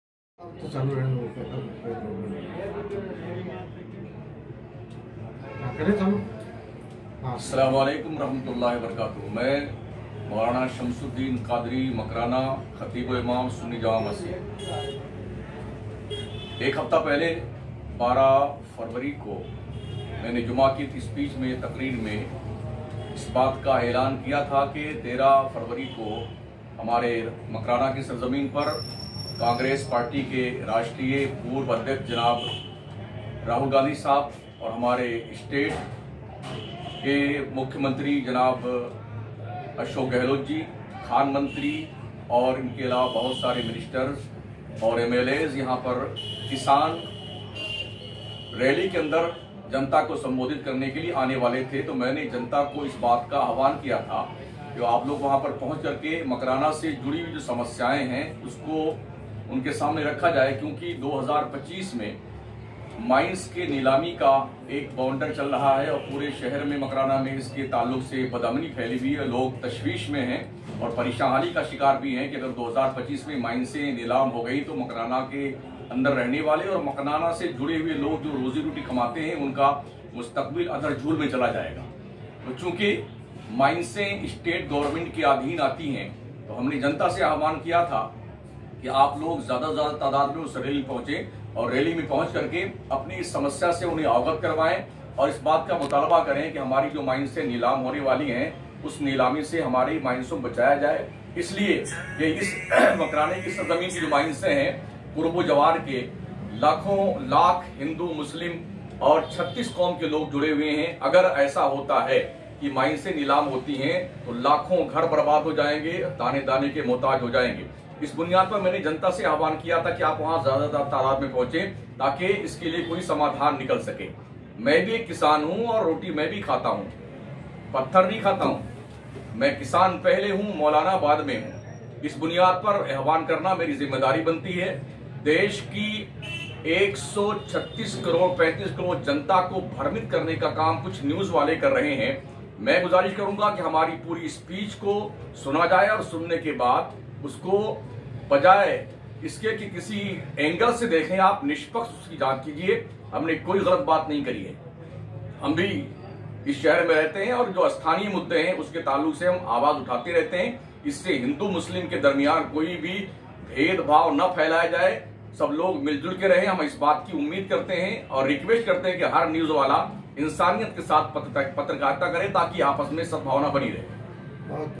Godi Media ke Aaropon ka muh tod Jawab By Moulana Shamsuddin Qadri Imaam Sunni Jama Masjid
जिस तरीके से गोदी मीडिया मकराना के मसले को गलत रंग देने की कोशिश कर रही है इसलिए इसे पढ़ना और शेयर करना जरूरी है जैसे कि सभी को पता है कि 13 तारीख को राहुल गांधी जी की तीनों काले कानूनों के विरोध में किसानों की महासभा नागौर जिले के मकराना शहर में हुई थी जो कि किसानों के समर्थन में थी और आज देश का हर वर्ग किसानों के समर्थन में खड़ा है उसी बात को लेकर मकराना की एक मस्जिद में इमाम साहब ने एलान कर दिया कि ज्यादा से ज्यादा किसानों के समर्थन में सभा स्थल पर पहुंचे और उन्होंने एक जुमला और बोला कि 2025 की तलवार हमारे सर पर लटक रही है उनका इस जुमले से मतलब यह था कि 2025 में राजस्थान की कानों की नीलामी होने जा रही थी जिस को रोकने के लिए सरकार को कई बार ज्ञापन भी दिए हैं जो कि प्रदेश सरकार के अधीन आती है इस सभा में कान मंत्री मुख्यमंत्री और तमाम मंत्री मौजूद थे इसलिए उन्होंने यह जुमला बोला था पर बड़े ही दुख और अफसोस के साथ कहना पड़ रहा है कि गोदी मीडिया के जी न्यूज़ ने इस मुद्दे को उठाकर राजनीतिक रंग देने की कोशिश की और उनके बोले हुए 2025 के जुमले को इलेक्शन से जोड़ दिया जबकि यह सच नहीं है और इस पर हिंदू मुस्लिम करने से बाज नहीं आया और अगर मुसलमान किसानों के समर्थन में मस्जिद से ऐलान करवाएं या हिंदू भाई मंदिर से ऐलान करवाएं या सिख भाई गुरुद्वारा से ऐलान करवाएं तो गलत क्या है हां हम सब कहते हैं और बड़े गर्व से कहते हैं हम किसानों के समर्थन में हैं किसान हमारे अन्नदाता है जय जवान जय किसान मेहरबानी होगी इस पोस्ट को ज्यादा से ज्यादा शेयर करें